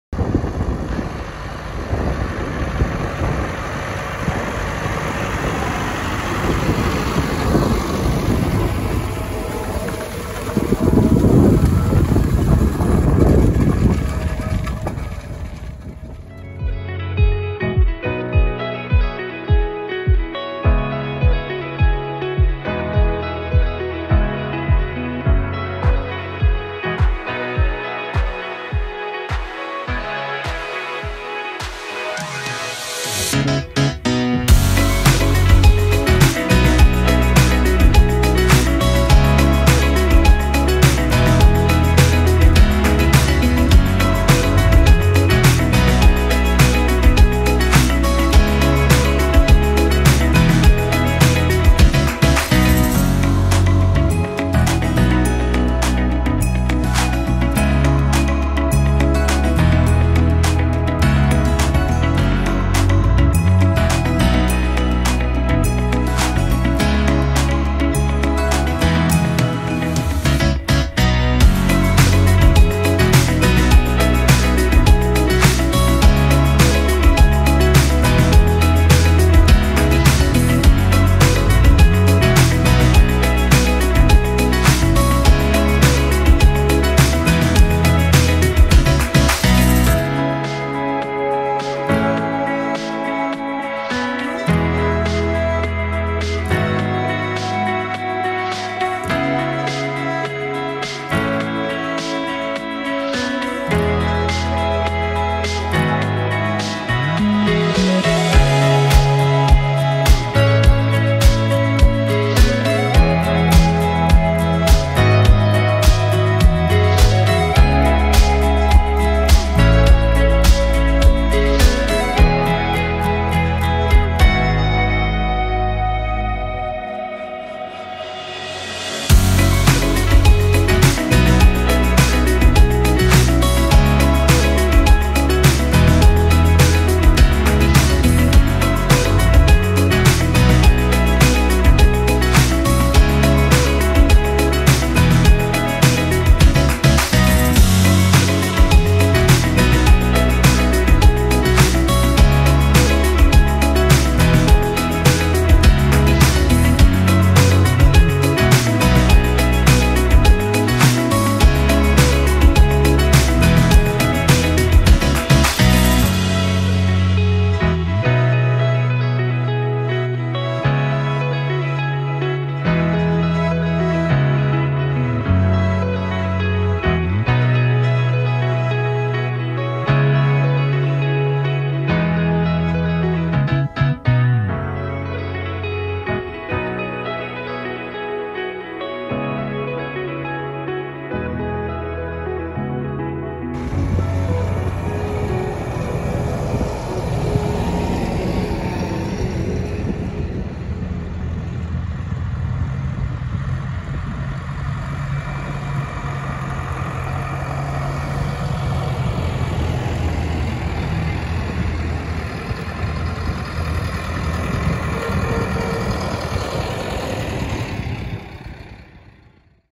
Lægning og såning af løg 2021 - Torup Bakkegård & Orelund
En lille video af lægning af stikløg og såning af såløg hos TBO i foråret 2021